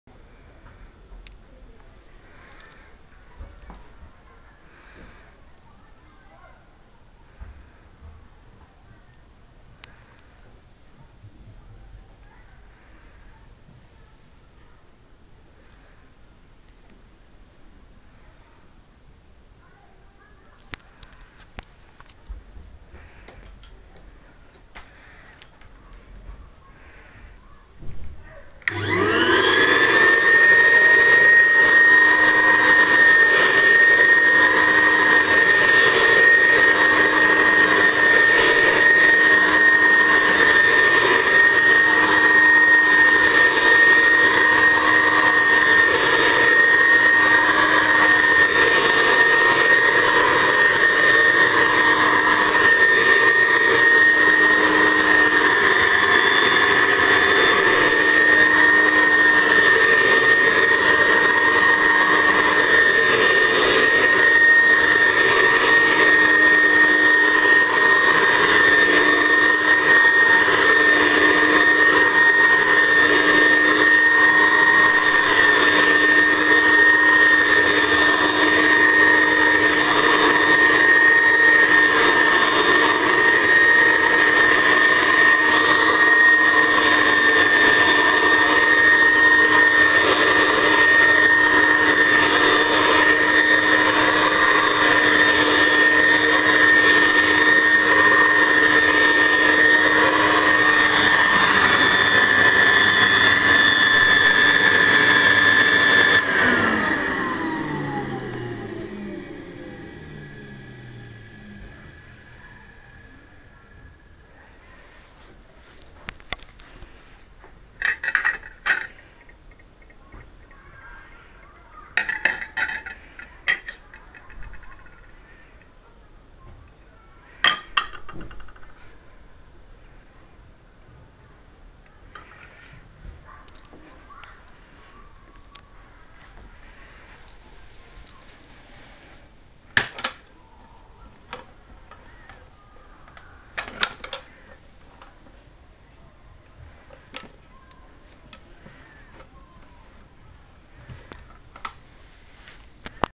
Philips HZ 4930 / 18A
Here we have this little Philips stick vac  in action, these machines were sold as the economic model along with another sticks vac and canisters of the era, this was one of the most populars machines in Spain back in the late 60s and the 70s.
It could be used as a stick vac for cleaning carpets and floors, and then you can detach the wand and the handgrip and use it as a hand vac to do the upholstery, and above the floor cleaning. It's quite powerful considering it has a small 325 watt motor and does a good job on rugs.

325 watt, 220 volt, 50hz, metal body, cloth bag. Made in Spain from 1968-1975.